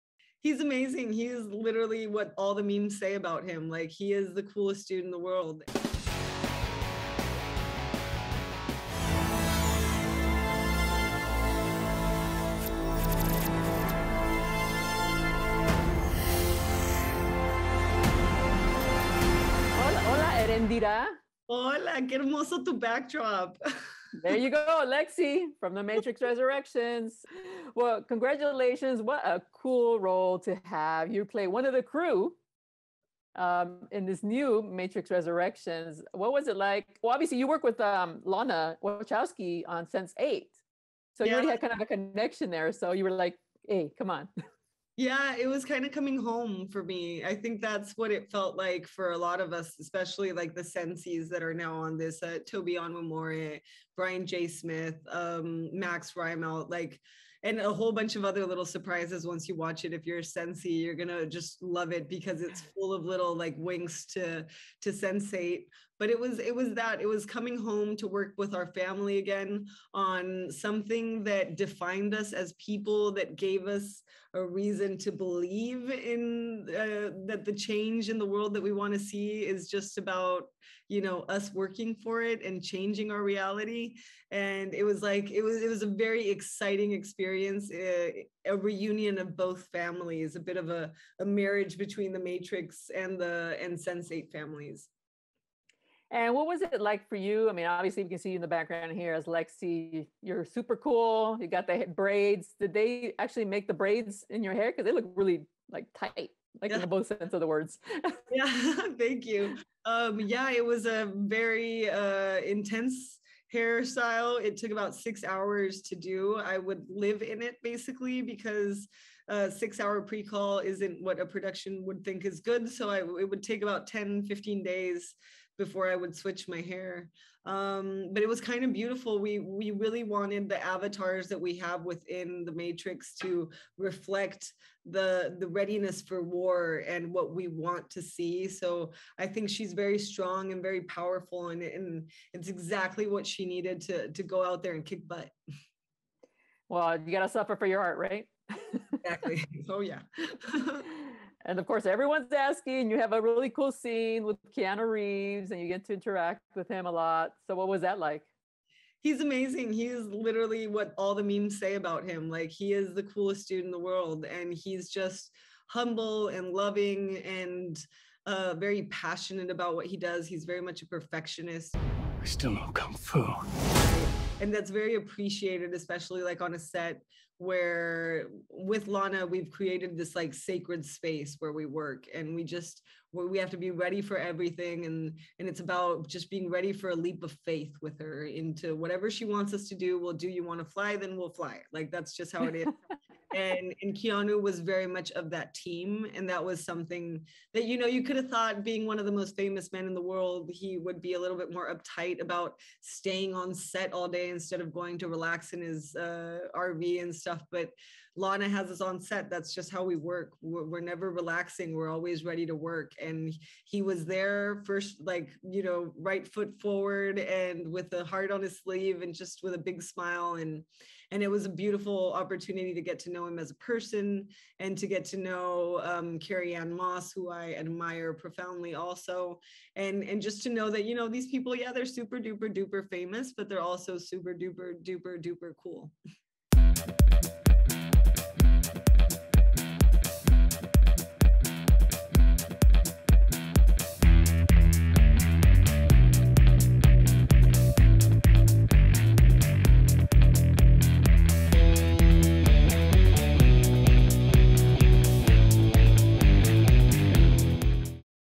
Keanu Reeves is the 'Coolest Dude in the World' Says Matrix 4 Costar Erendira Ibarra
Lana Wachowski recruited his Sense8 cast for Matrix Resurrections including Erendira Ibarra. We talked to her about starring with Keanu Reeves who she calls the "coolest dude in the world" and a perfectionist on set. CineMovie chats with the actress about playing "Lexy" in the fourth follow-up.